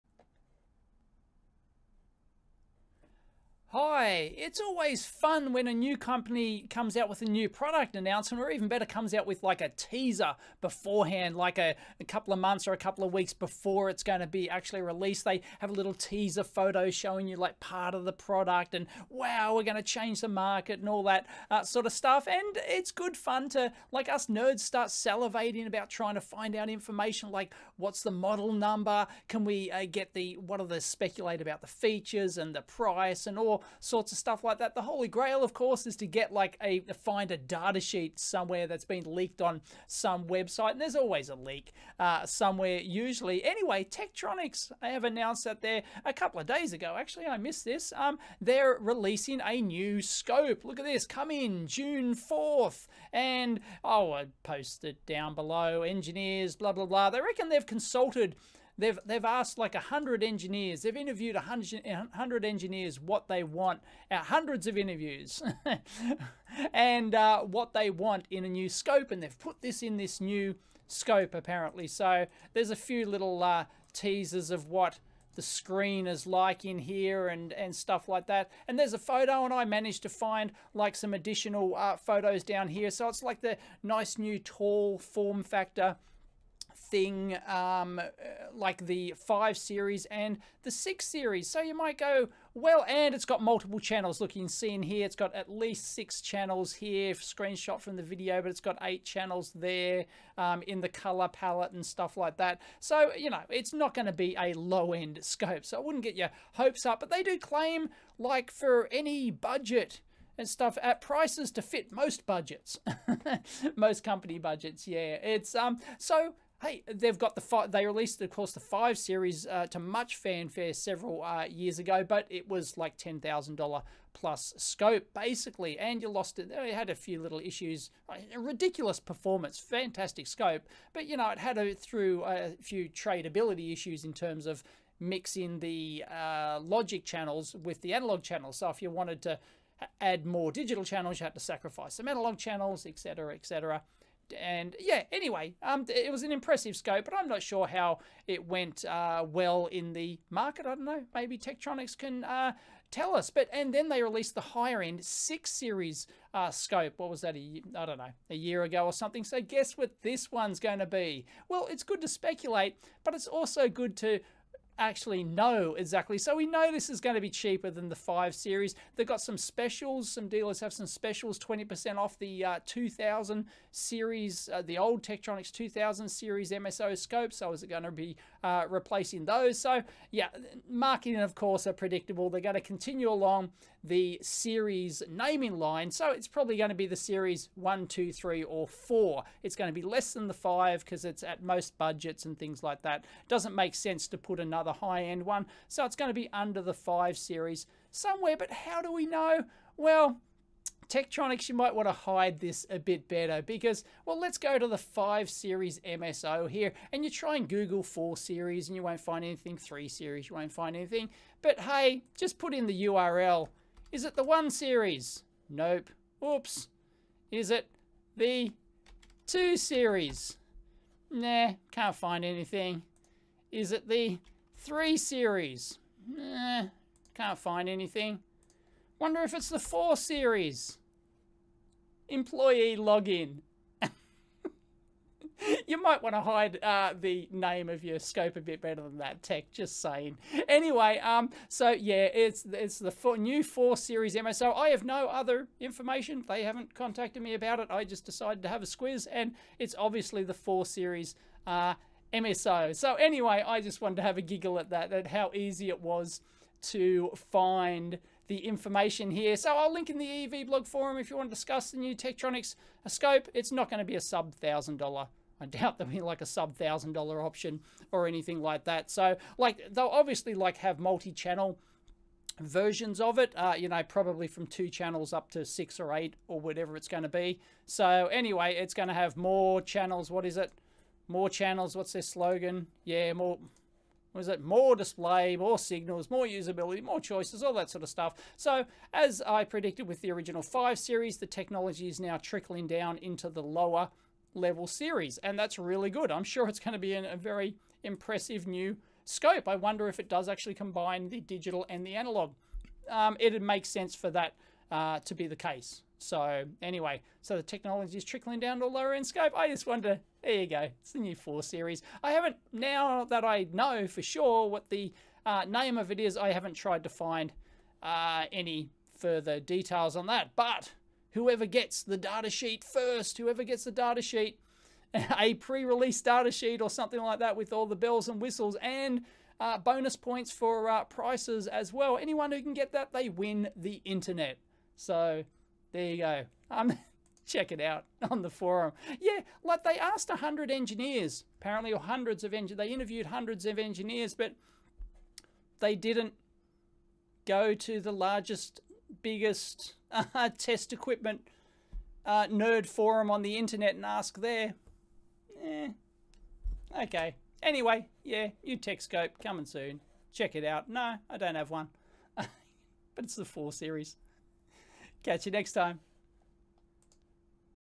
Let's Play Guess The New Tektronix Oscilloscope
Dave shows how it's not that hard to get product pre-release info, in this case tracking down the model name of the new Tektronix scope to be released on June 4th.

Litecoin: MRnSifHf1jjgimgFq56YWXb3uo1iHdhg1R
Ethereum: 0x11AceA38DCA9DbFfB4F35f3F746af65F9dED28ce
Ripple: rBgnUKAEiFhCRLPoYNPPe3JUWayRjP6Ayg